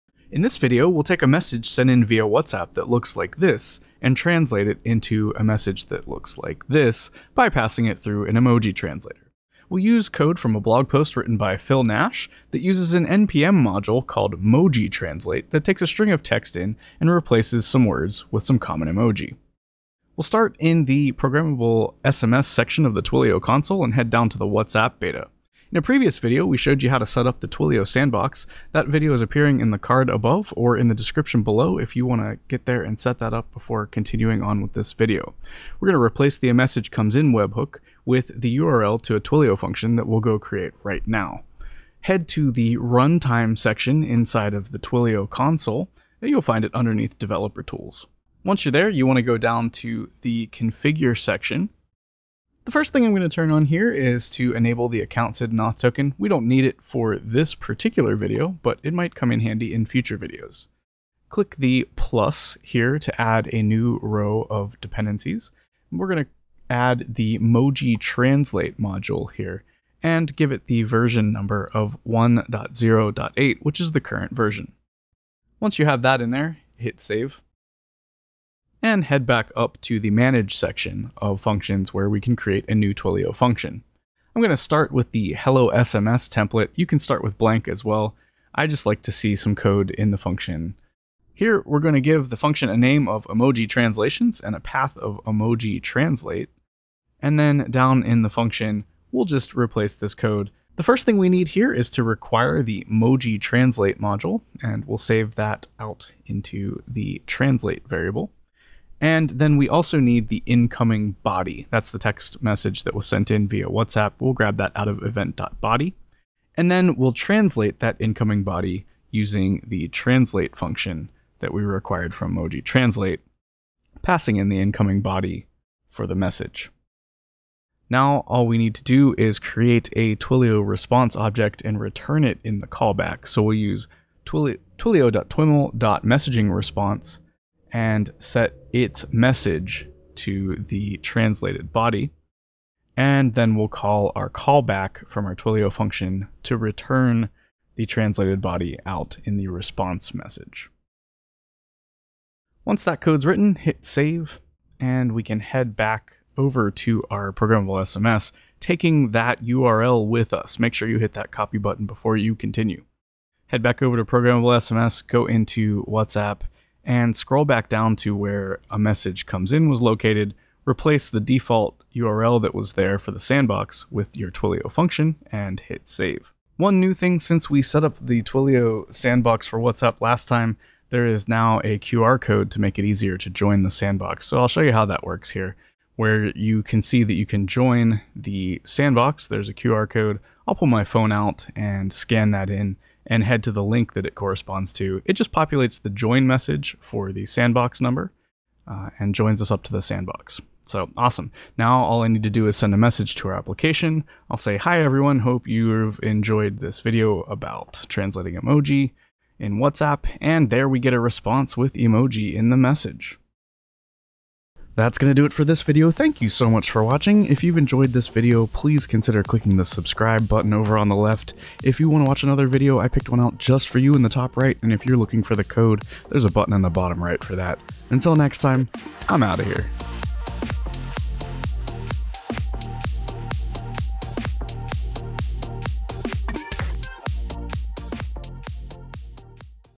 👉 Emoji translations with the 📞 Twilio API for 💬 WhatsApp and Node.js
Translate incoming WhatsApp messages into emoji with the Twilio API for WhatsApp and the moji-translate Node.js module. All it takes is a simple Twilio Function hooked up to your incoming WhatsApp webhook. Learn how in this 4 minute movie.

Watch the Getting Started with the Twilio API for WhatsApp video to get your Sandbox set up.